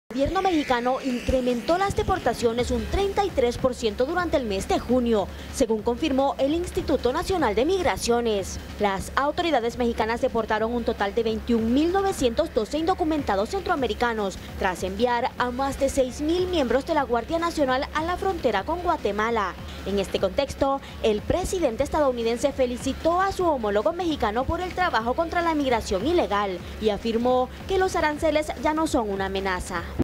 Crecen las deportaciones de centroamericanos en México
En más noticias internacionales, crecen las deportaciones de centroamericanos en México.

Miércoles 03 de julio de 2019